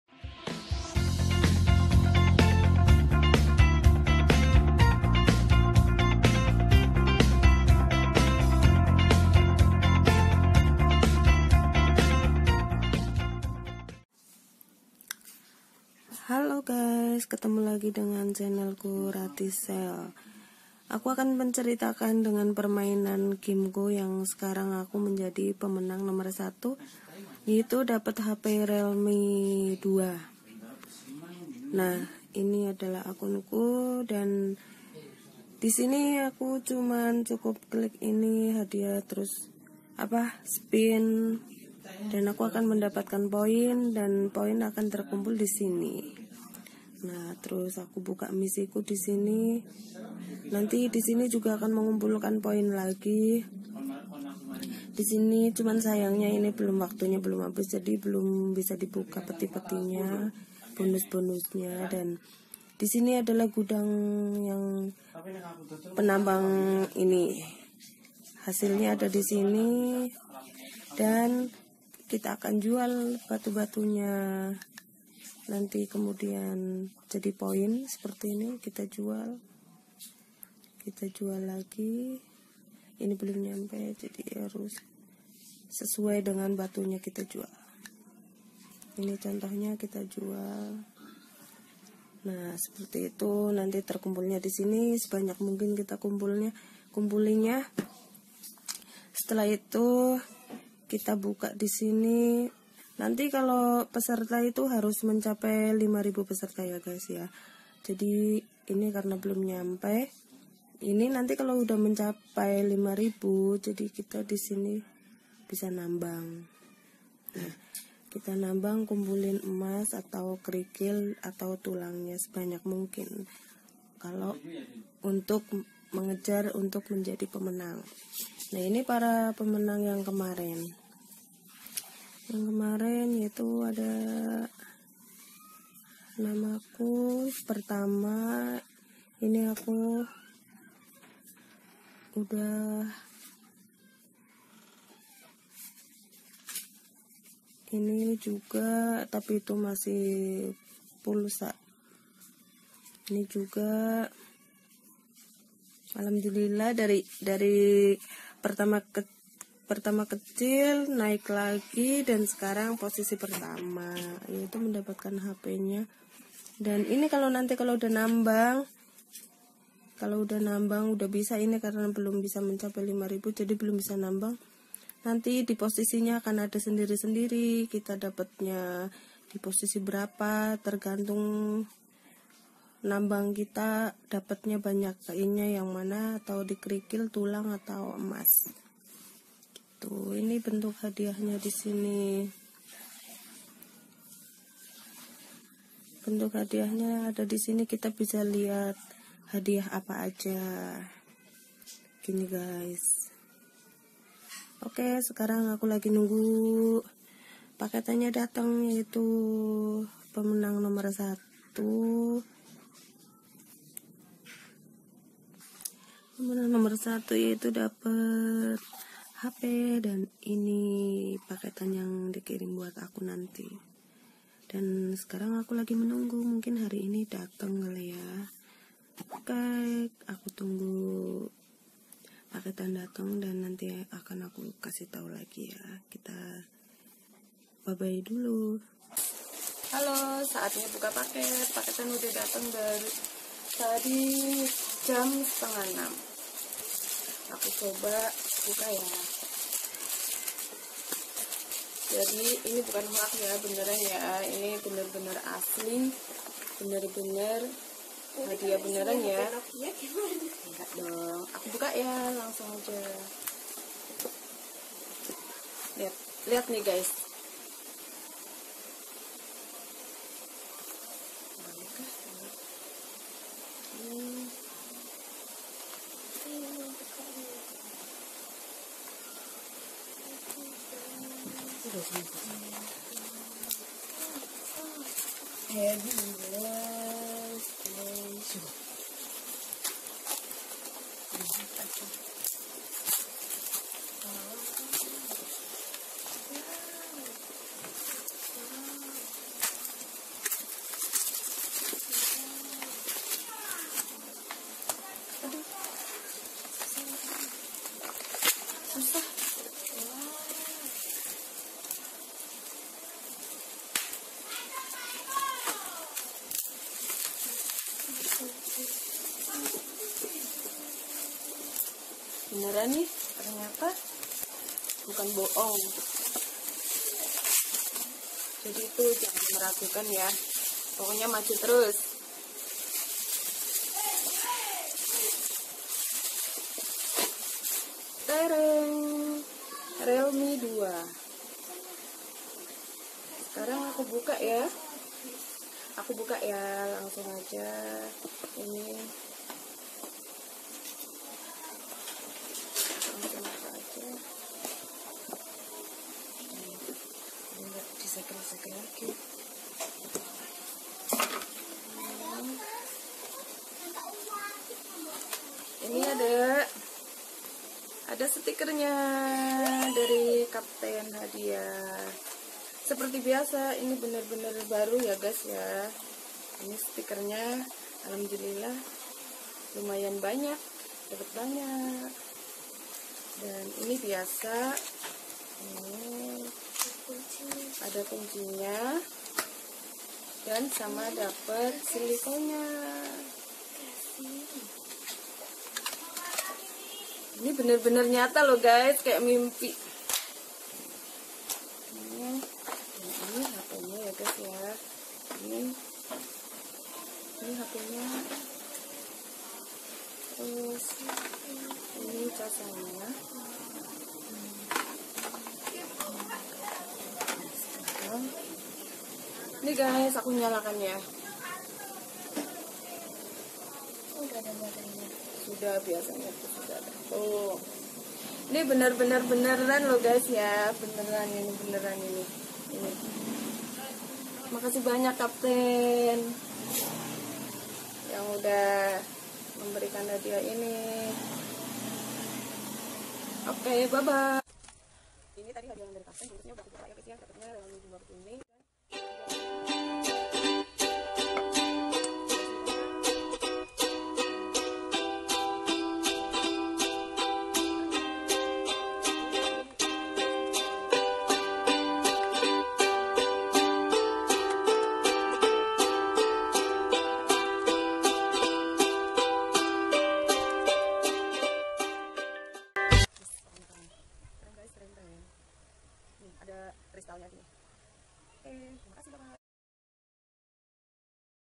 Dapat hp gratis-dari game
Banyak game"yang udah aku coba,, tapi belum pernah dapet hadiah..
Tp untuk game yg ini aku sering mendapat kanya.. Kali ini jadi juara yang pertama dapetin hp,, tentunya aku senang sekali,, main game tp ada hasil nya,,